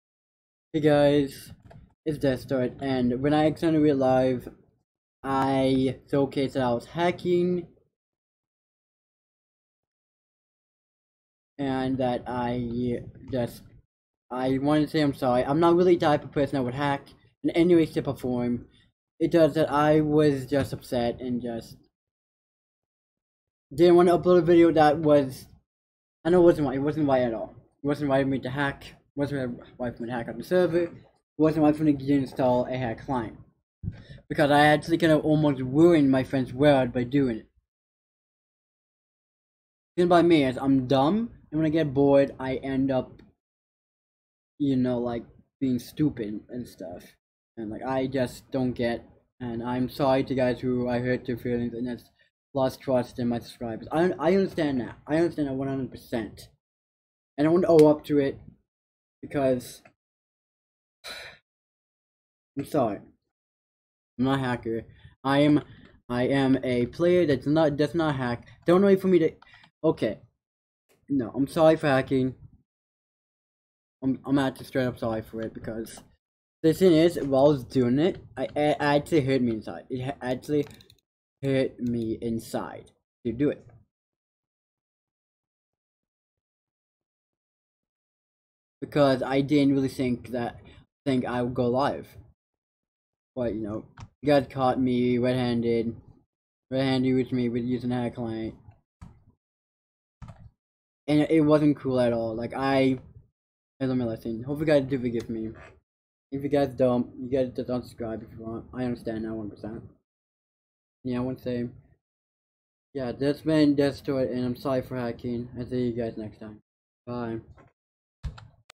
I am so sorry for hacking
I am so so sorry for hacking that I will never ever hack again and I hope to gain all of you trusts back if not I understand 100% why.